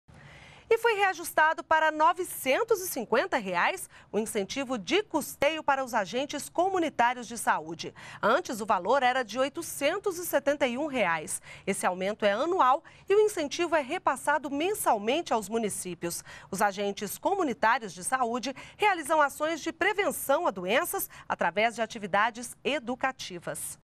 Incentivo para agentes de saúde aumenta para R$ 950
NBR NOTÍCIAS - 25.02.13: Nesta segunda-feira (25), o incentivo de cuteio para os Agentes Comunitários de Saúde (ACS) passou de R$ 871 para R$ 950. Esse reajuste é anual, e o incentivo é repassado mensalmente aos municípios. Os Agentes Comunitários de Saúde são responsáveis pelas ações de prevenção a doenças, através de atividades educativas feitas nas comunidades.